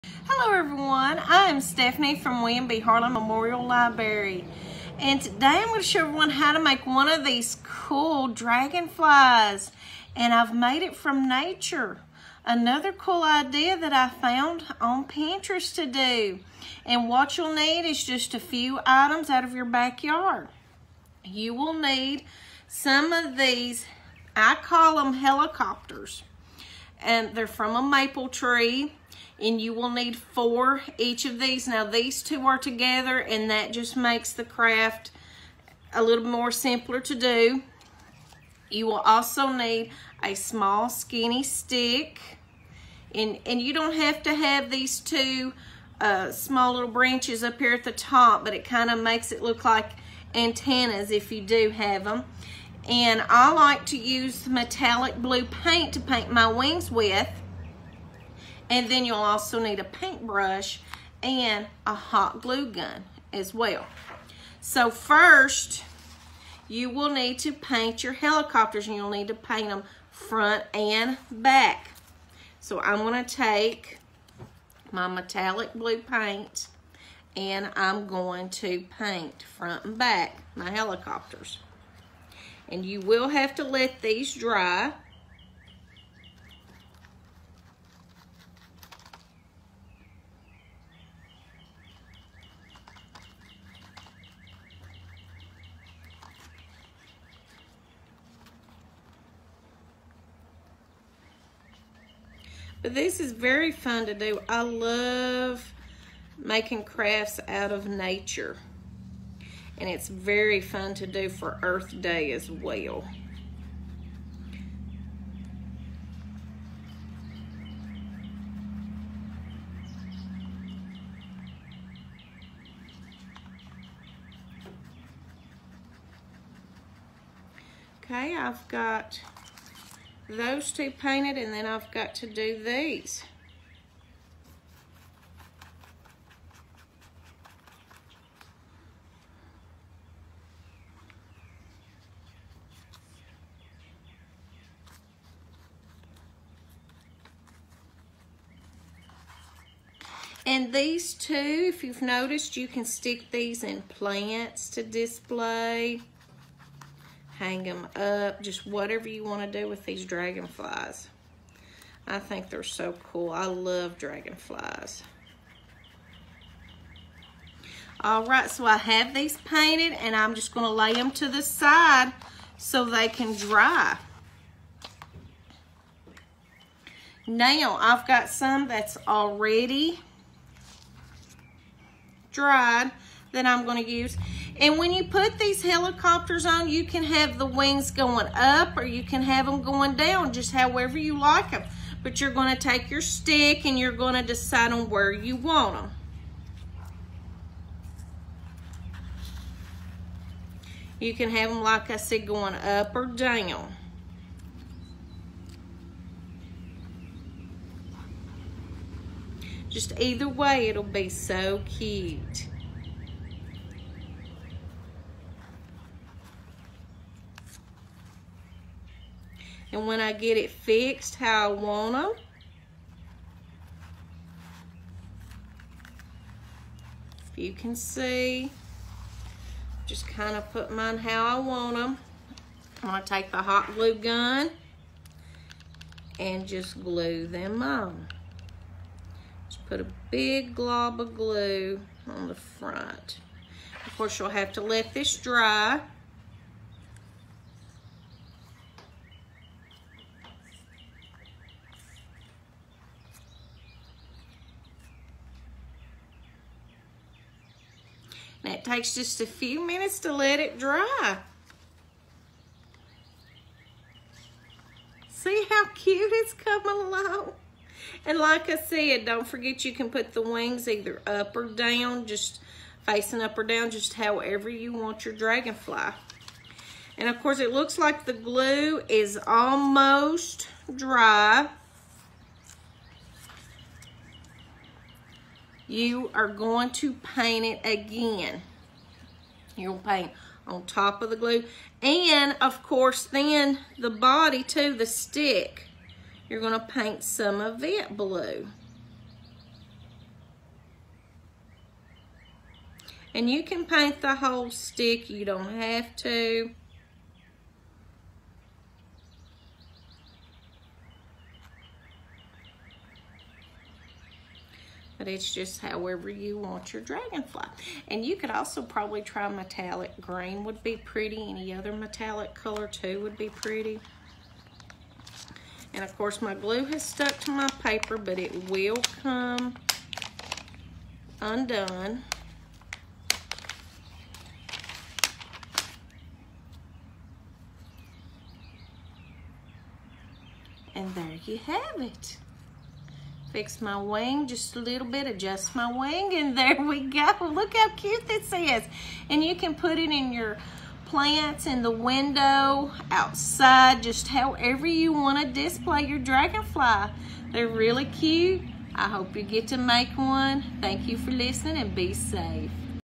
Create with WBHML: "Whirlygig" Dragonflies with Ms. Stephanie
On this episode of Create with WBHML: Ms. Stephanie has a family-fun Earth Day craft  that uses maple tree seeds (aka Tree Spinners, Whirlybirds, Helicopters) to make cute dragonfly decorations. She calls her creations "Whirlygig" Dragonflies.

Materials:
• 4 Maple Tree Seeds
• Metallic Blue Paint (of paint of your choice)
• 1 Small Stick
• Paint Brush
• Hot Glue or E6000

Crafting Level: Adult supervision with glue